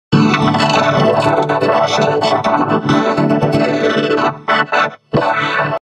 {FTU/KYOOBUR9000'S 11TH ANNIVERSARY SPECIAL} An Audio Effect I'll Call "Free Spirit Mega Ensemble"
FTU = FORGOT TO UPLOAD

(A.K.A. : Klasky Csupo Robot Logo Enhanced With Free Spirit Mega Ensemble)

and Happy 11th Anniversary to You, @Kyoobur9000! ;)